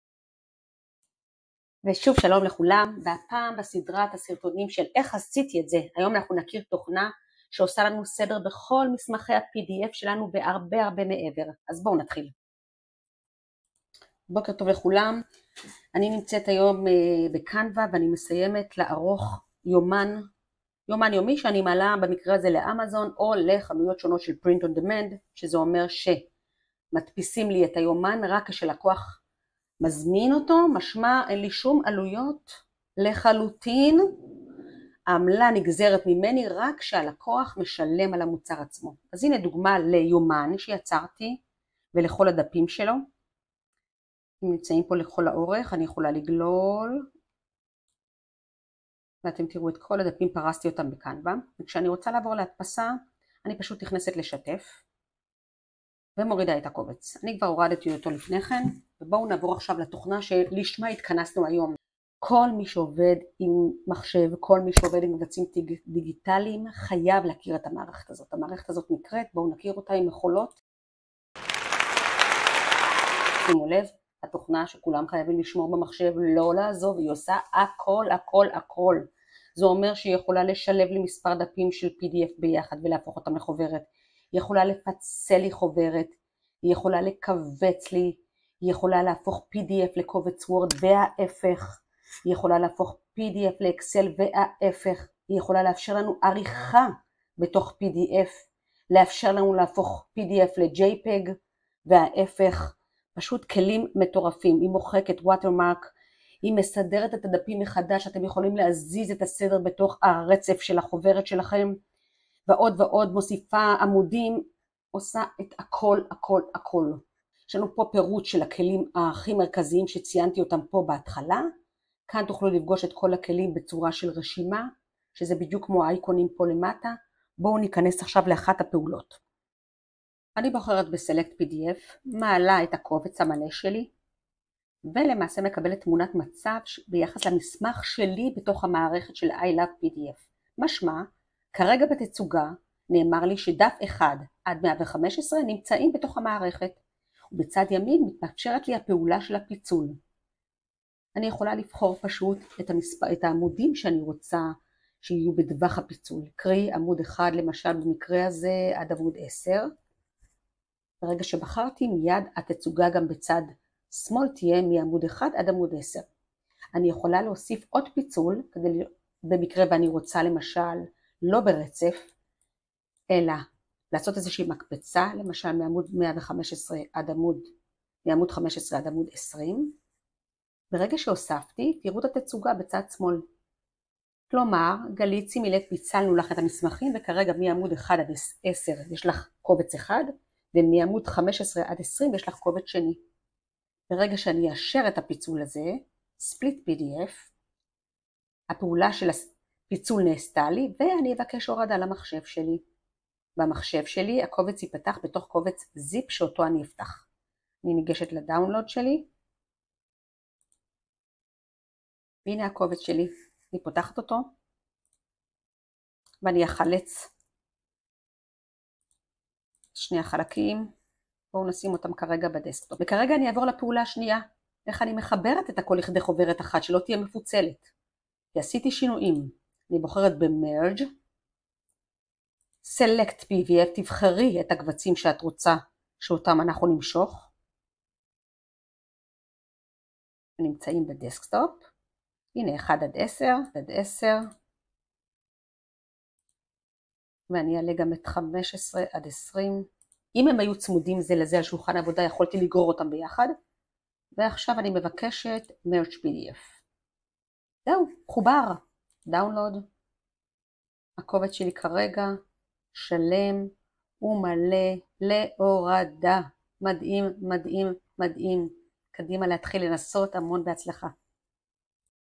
i love pdf איך עשיתי את זה ?
התוכנה המושלמת לניהול כל המסמכים .
מגוו של כלים מטורפים שיאפשרו לכם, לפצל קבצים, לקבץ, לעמד, לערוך, לסדר מחדש ומה לא.